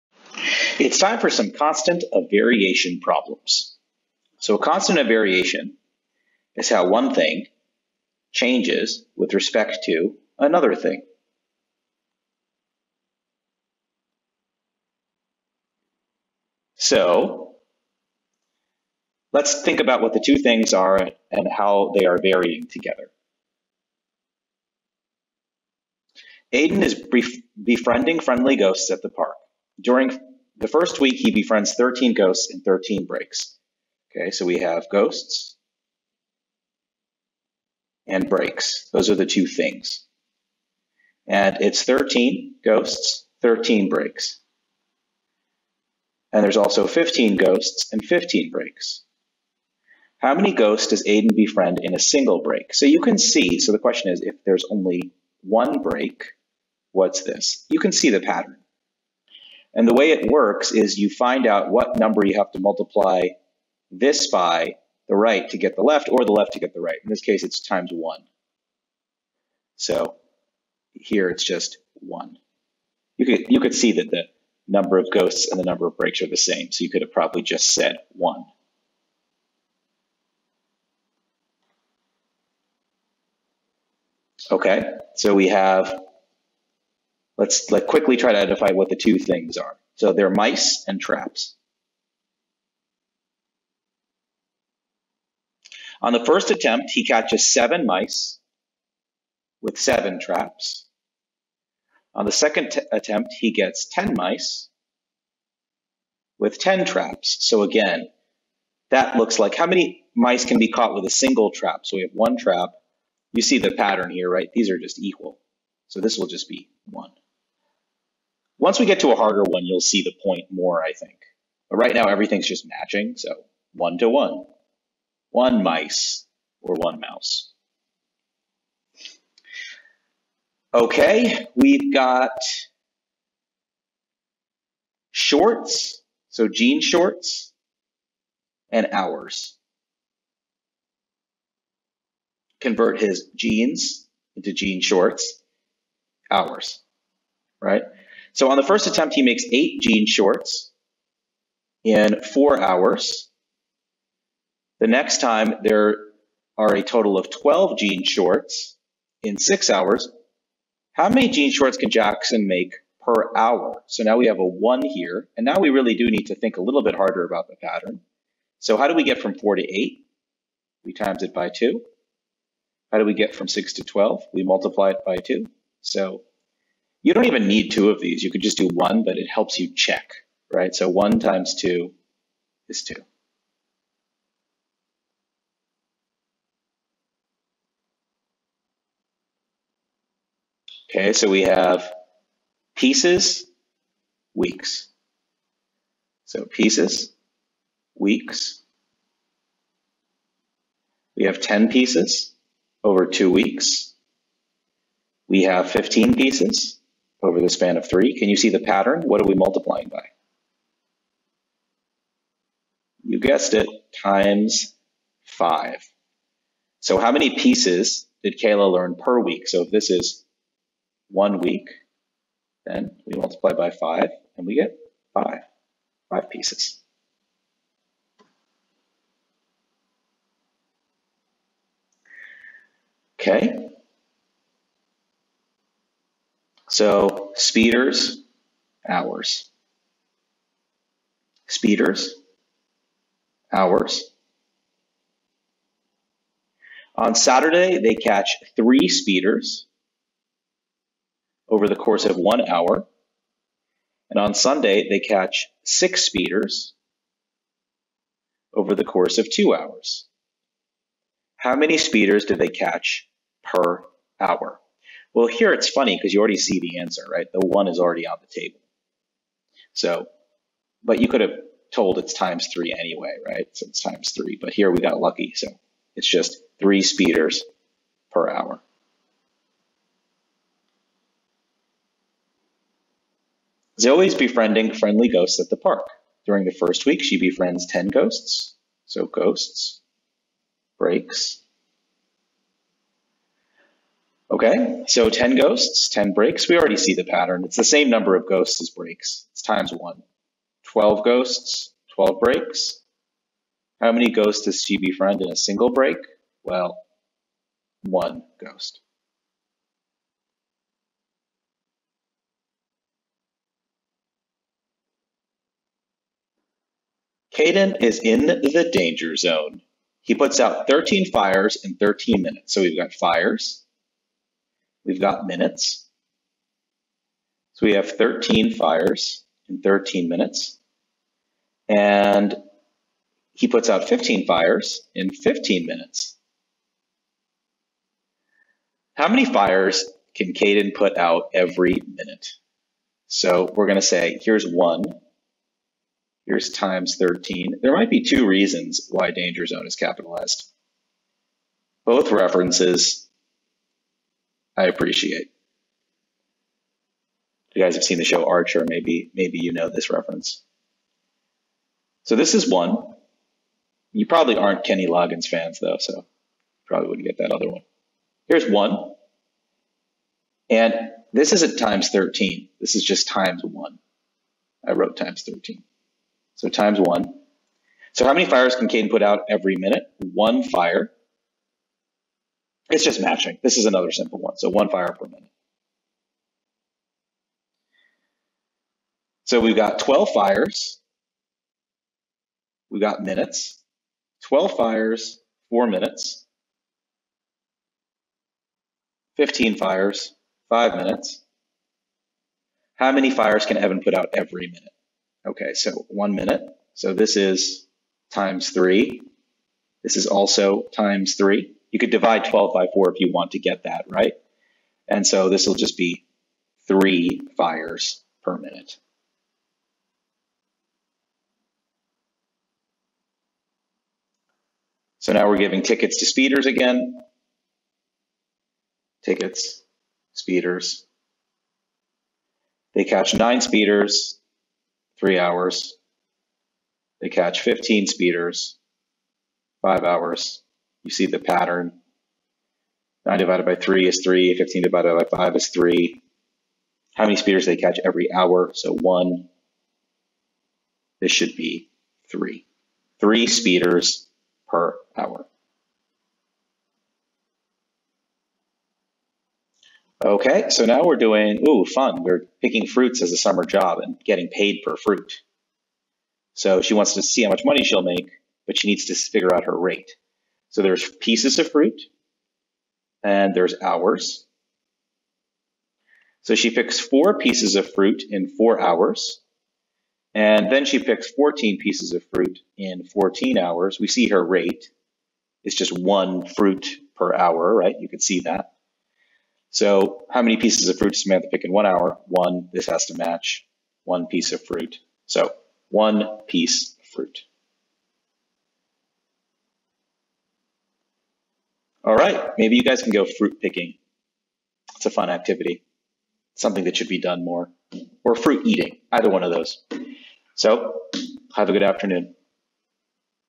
7.26 Constant of Variation
Constant of Variation
Determine the constant of variation from a word problem.